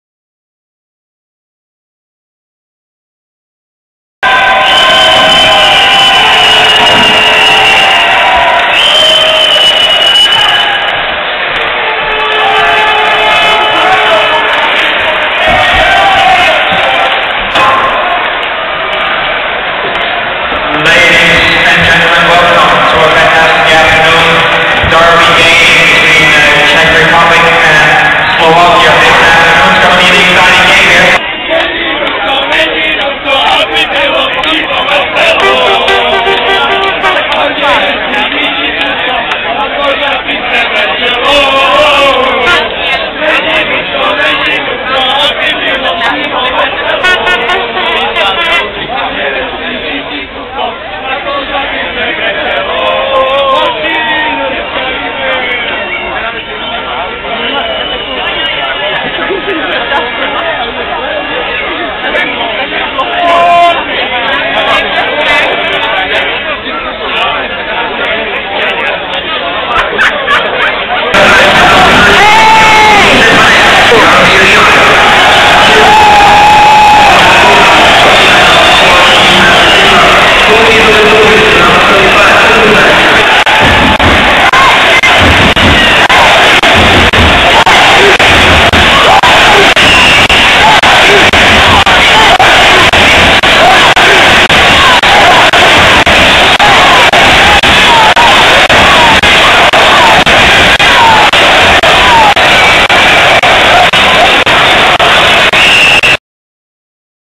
Mistrovstvi sveta v hokeji Kloten- videa
Sestrih kratkych videi--cr-dansko.cr-finsko,cr-slovensko